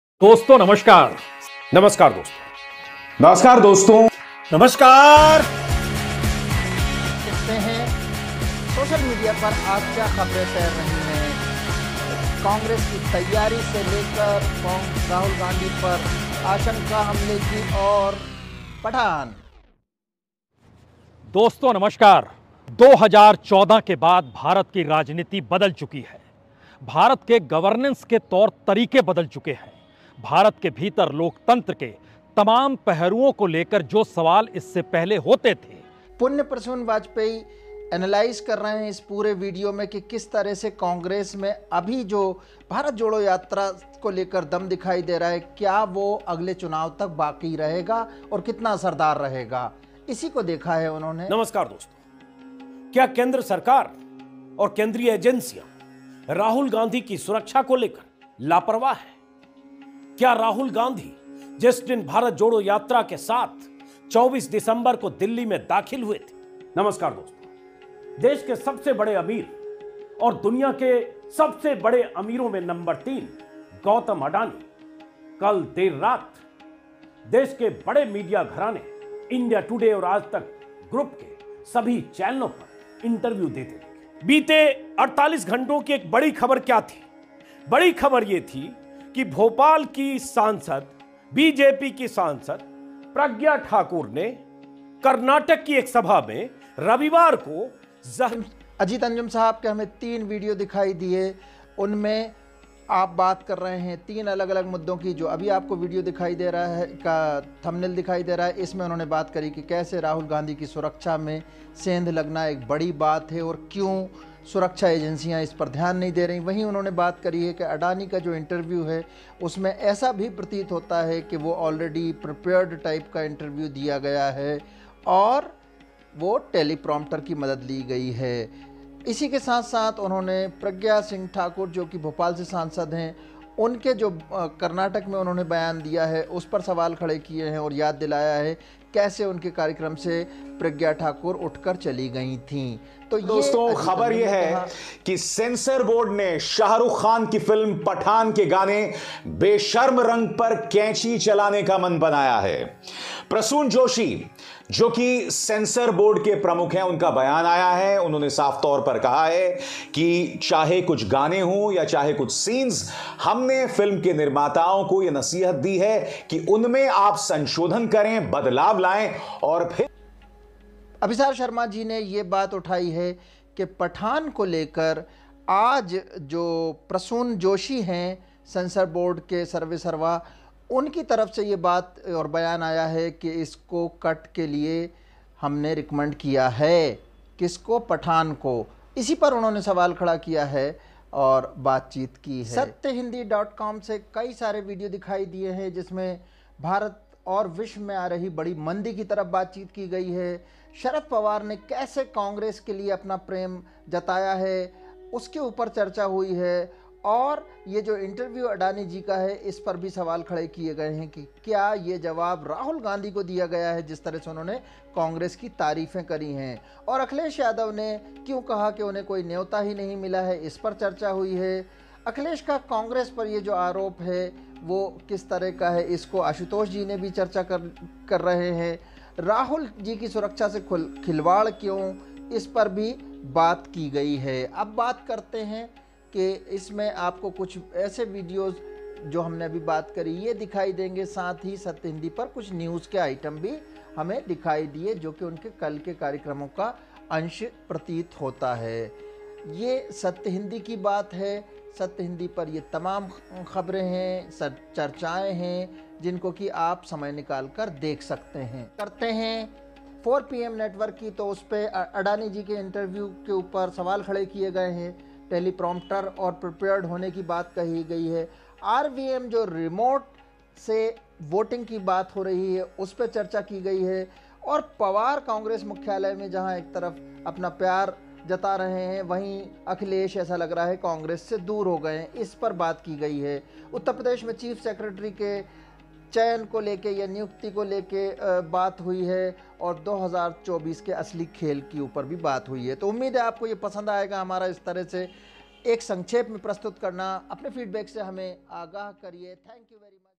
29 December यूट्यूब चेनलों पर चर्चा YouTube channel | social media news | social media news updates
सोशल मीडिया खास तौर से यूट्यूब चेनल पर 29 दिसम्बर को क्या हुई चर्चा ? Know what has been  discussed in social media on 29 December

*This Video is strictly for Legal Awareness & Education Purpose only.